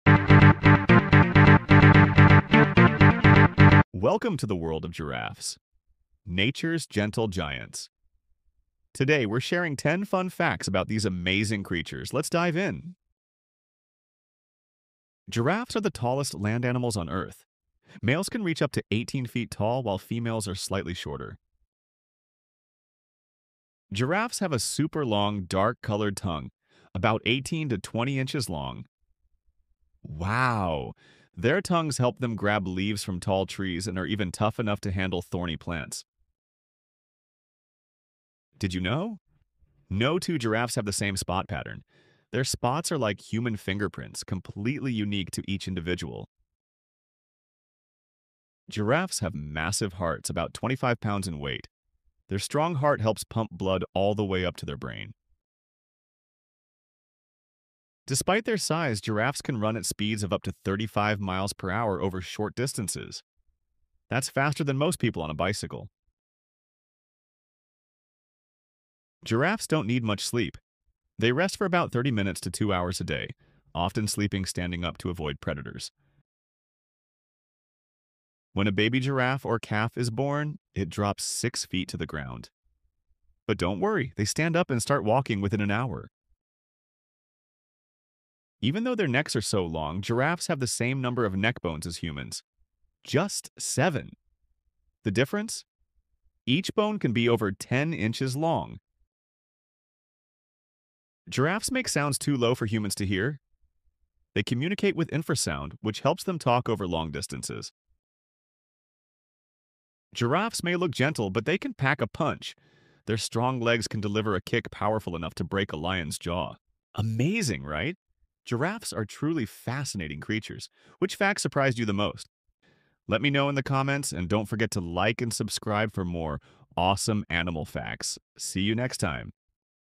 10 Fun & Educational Facts About Giraffes
Giraffes are some of the most fascinating animals on the planet! In this video, we explore amazing facts about these towering creatures—how they survive in the wild, their unique adaptations, and why they have such long necks. Did you know giraffes only need about 30 minutes of sleep per day? Or that their tongues can be up to 20 inches long? Watch to discover even more incredible giraffe facts!